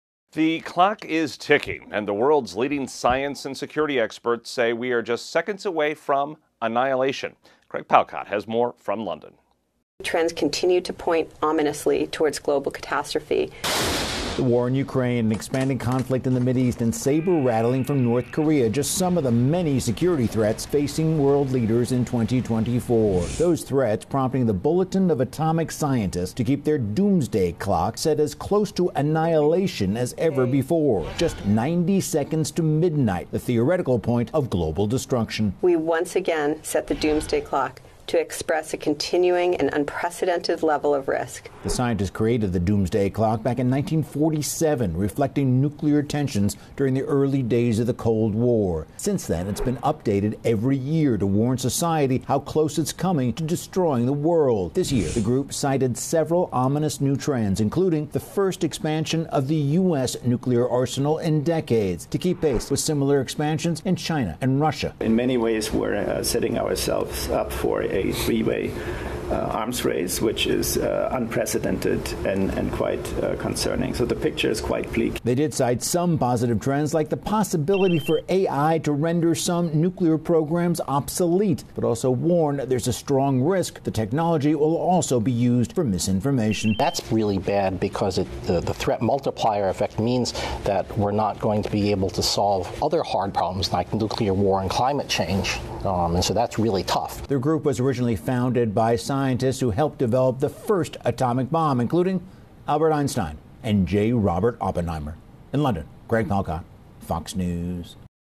Doomsday Clock at 90 Seconds to Midnight Amid Global Threats
The clock is ticking and the world's leading science and security experts say we're just seconds away from annihilation.